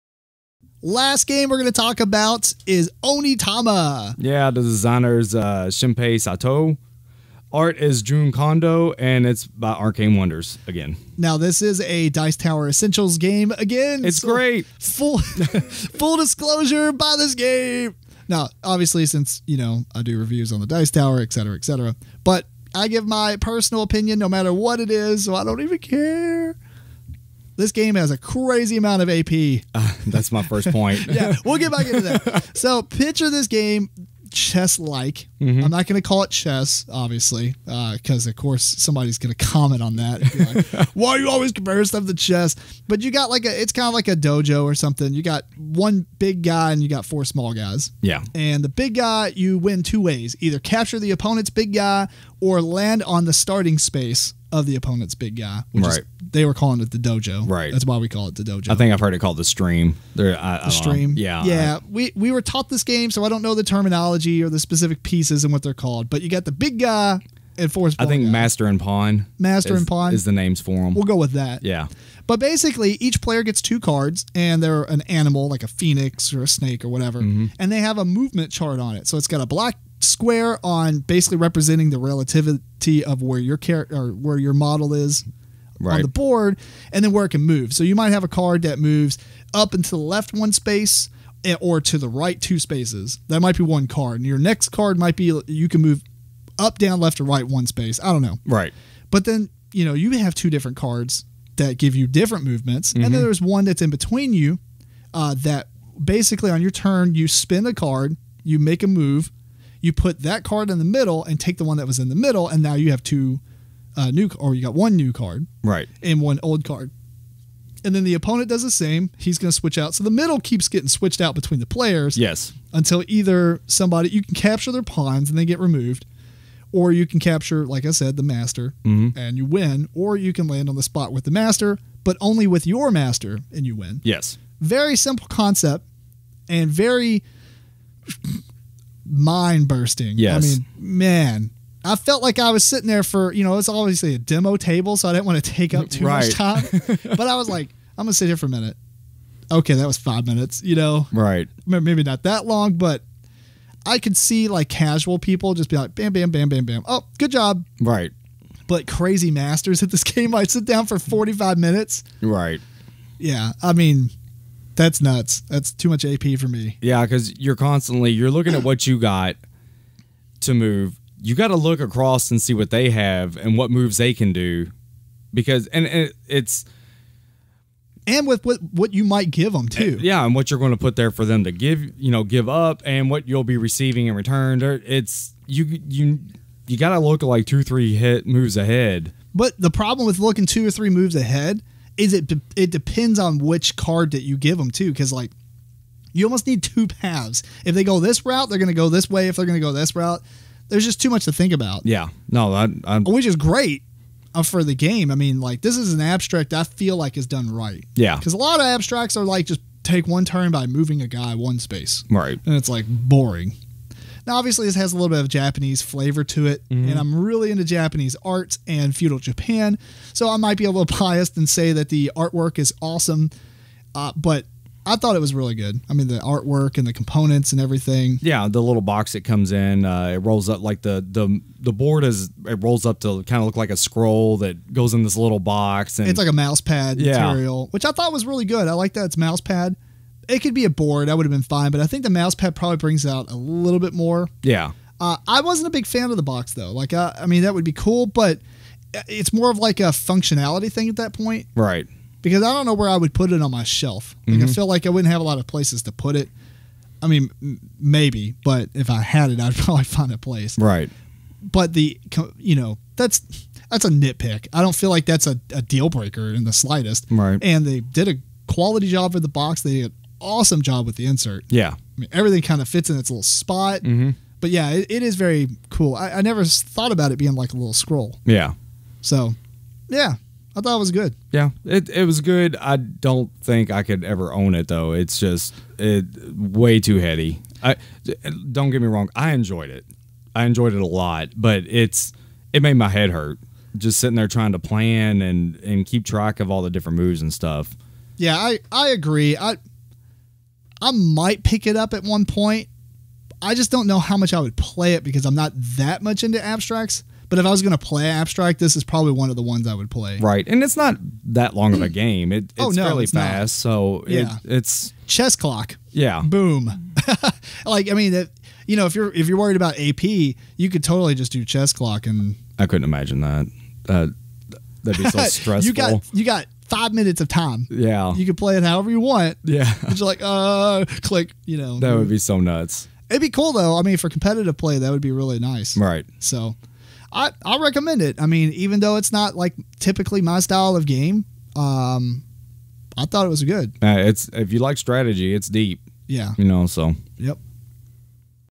MeepleCore Podcast EP8 - Onitama
Tim Jennette (Metal Meeple) and Matt RoBear talk about their first impressions of the board game Onitama

Designer: Shimpei Sato
Artist: Jun Kondo
Publisher: conception, Arcane Wonders, minimalGames